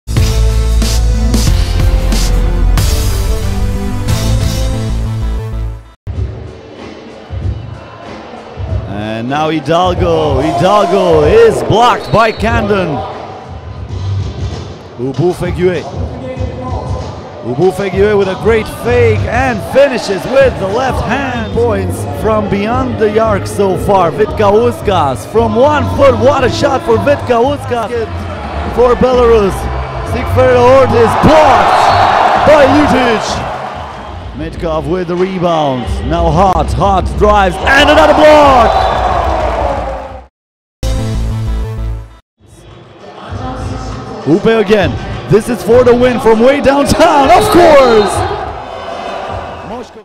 Top 5 Plays - Day 2 - 2015 FIBA 3x3 U18 European Championships - Minsk
Watch the top 5 plays from day 2 at the 2015 FIBA 3x3 U18 European Championships in Minsk!